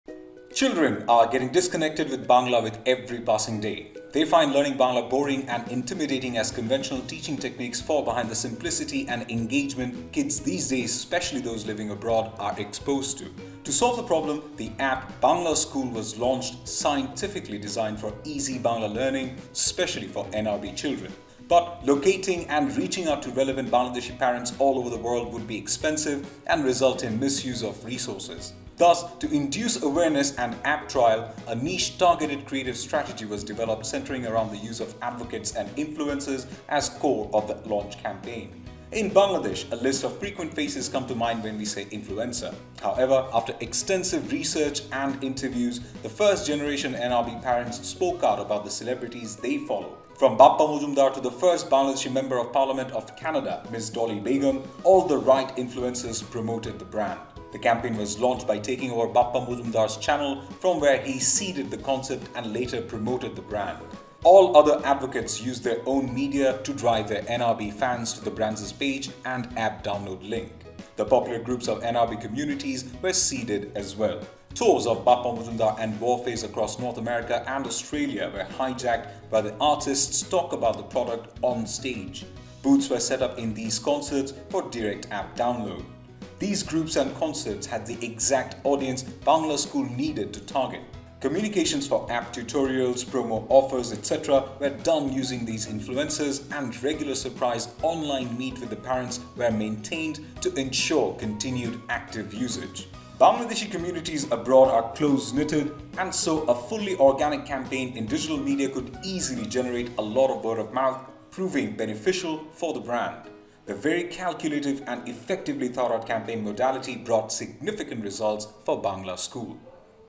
Bangla Iskool | User Acquisition Using Influencer Campaign | Case Video
Bangla Iskool is an app targeted towards the parents of children living abroad who wants to teach Bangla Language to their kids. Since these parents are located in a vast area around the world, it is not viable to promote using general media buying. Thus a smartly strategised influencer campaign was devised to reach and aware the TG. This lead to subsequent leap in app download and student registration.